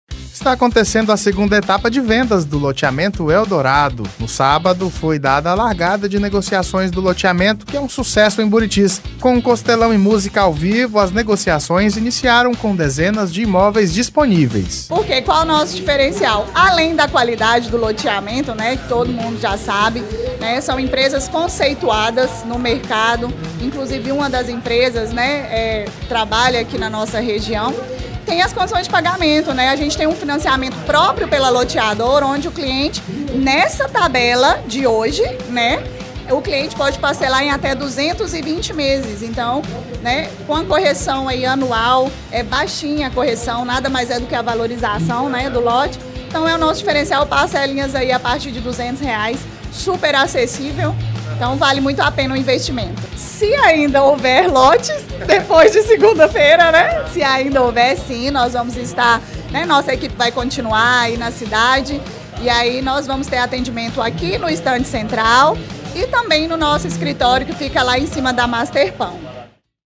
Loteamento Eldorado: Etapa 2
Está acontecendo a segunda etapa de vendas do Loteamento Eldorado. No sábado, foi dada a largada de negociações do loteamento que é um sucesso em Buritis.

Com costelão e música ao vivo as negociações iniciaram com dezenas de imóveis disponíveis